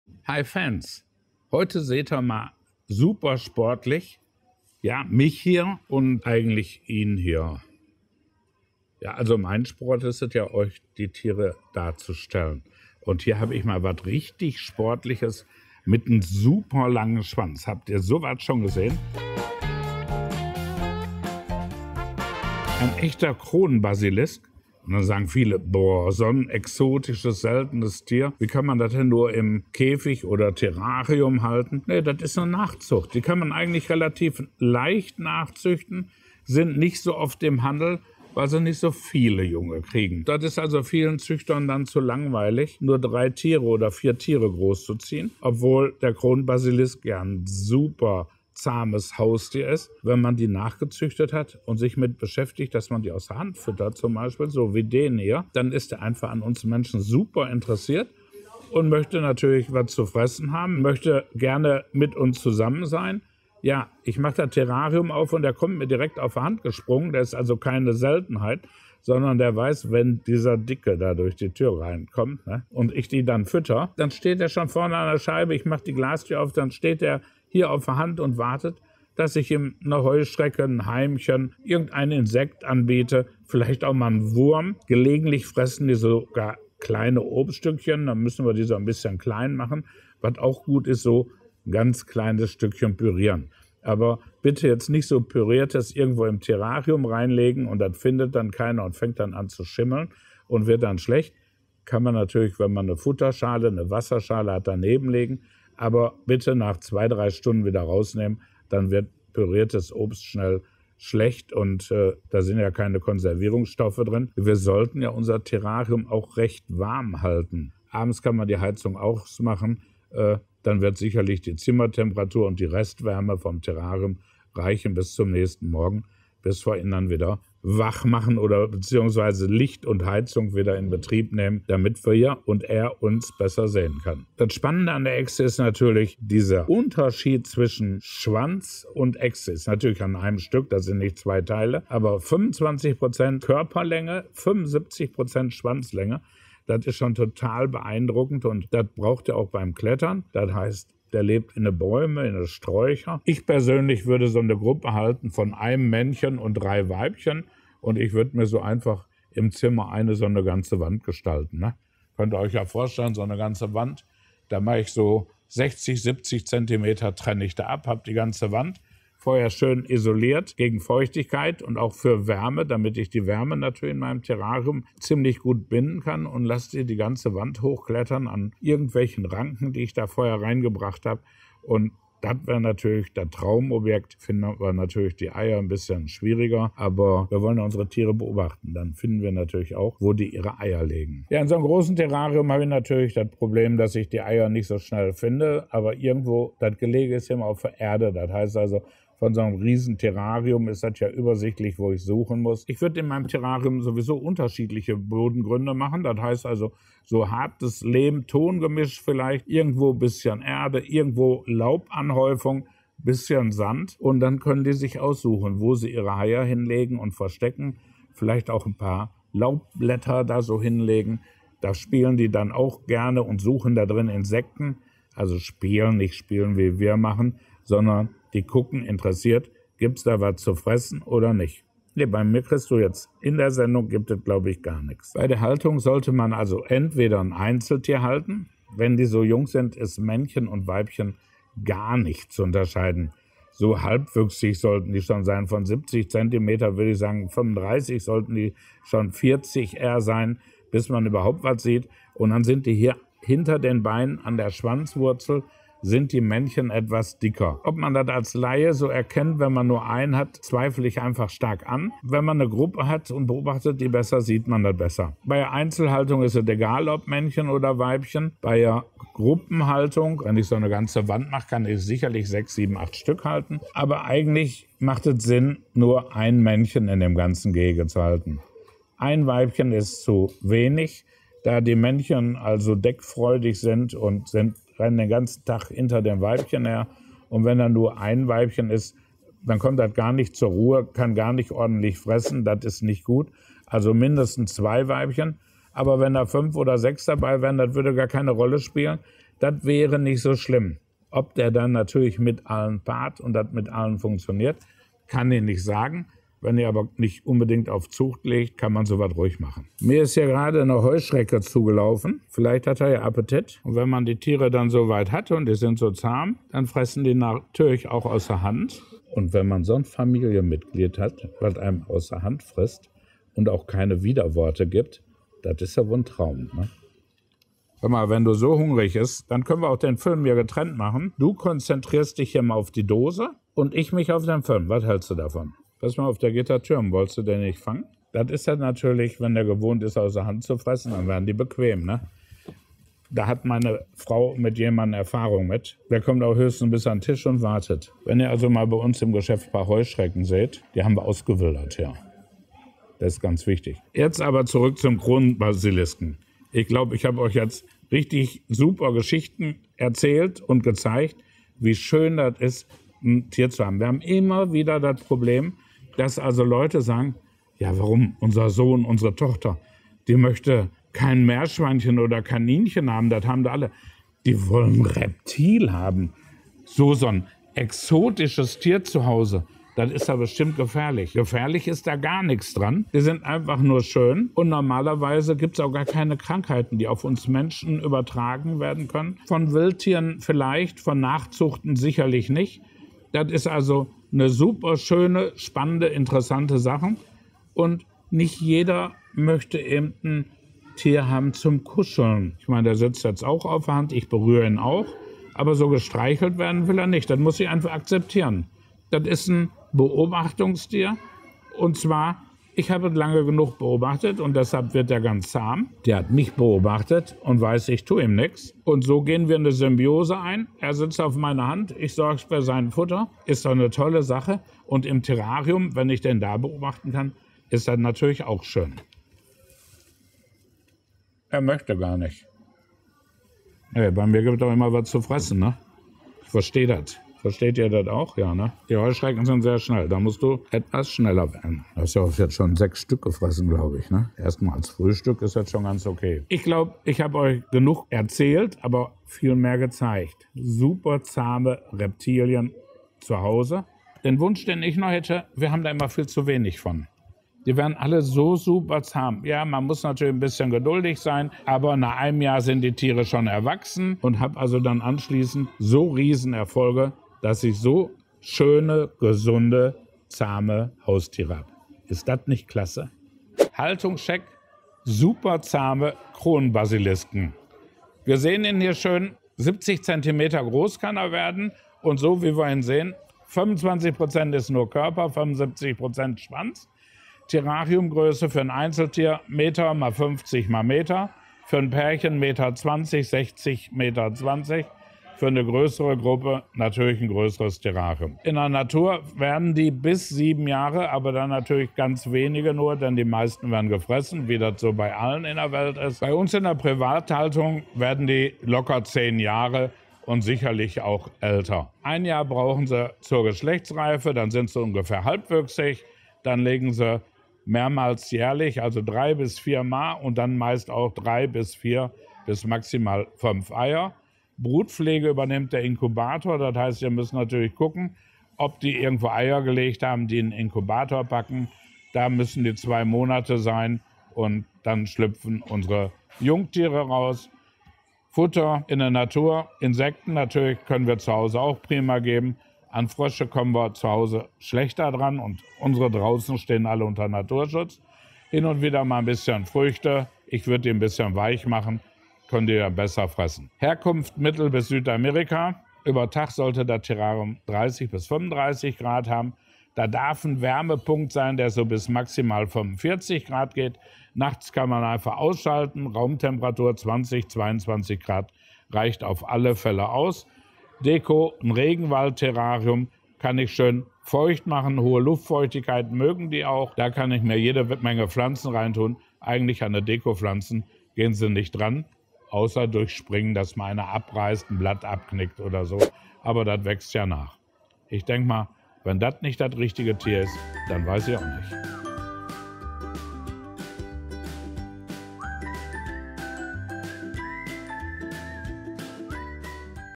KRONENBASILISK | NORBERT VLOGGT | Zoo Zajac
Ein Sprung und weg ist er! Nein, Spaß, aber sportlich ist er der Kronenbasilisk (lat. Laemanctus longipes). Norbert erzählt euch alles, was ihr über ihn wissen müsst.

Zoo Zajac GmbH
Konrad-Adenauer-Ring 6
47167 Duisburg